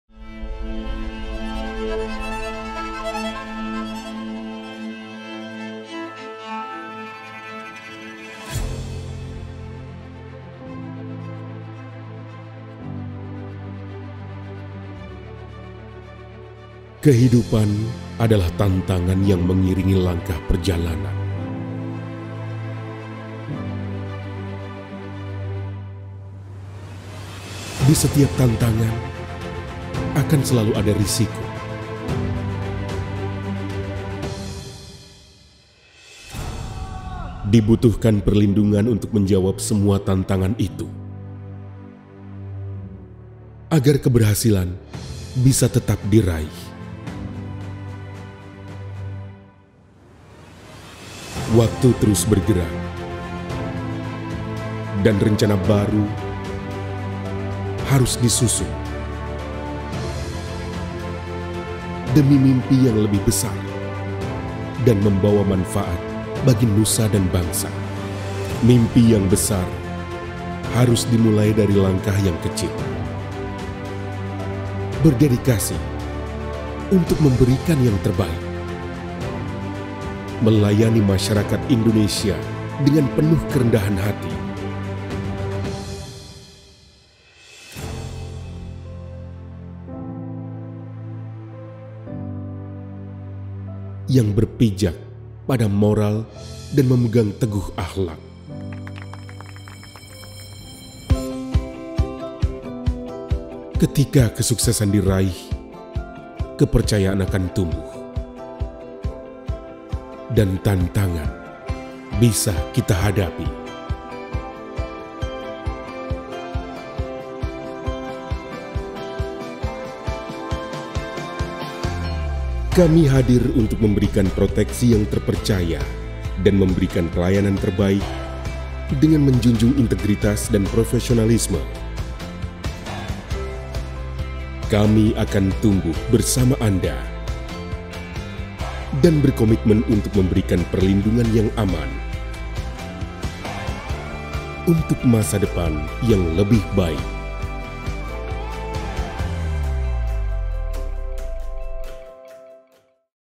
IFG Life - Company Profile
IFG Life siap menjawab tantangan!

Perjalanan baru telah dimulai, waktu terus bergerak, dan rencana baru harus disusun.
IFG Life berkomitmen untuk tumbuh bersama, menghadirkan asuransi jiwa yang modern, kredibel, prospektif, dan akuntabel, demi masa depan yang lebih baik.

IFG Life, Protecting Life's Progress.